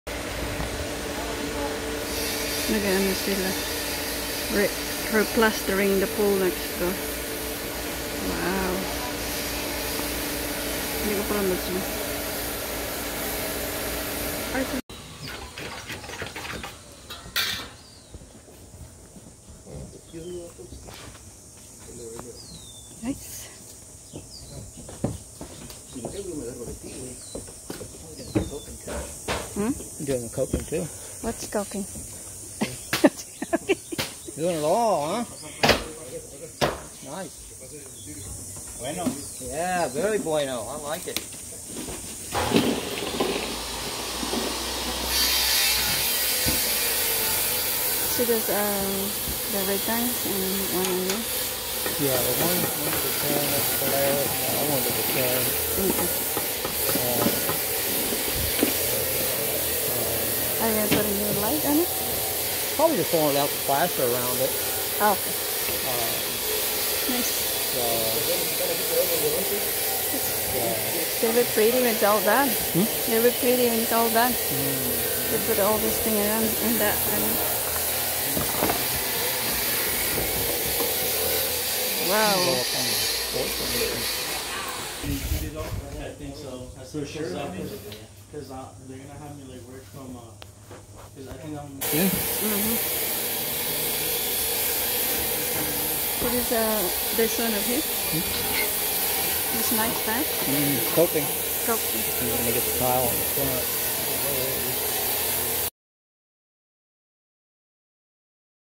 Preparation, Replaster a Pool
already drained water 💧 removed debris and dirt that may have accumulated on the surface. it also thoroughly cleaned and inspected for cracks, leaks and other damage that may need to be repaired before plastering.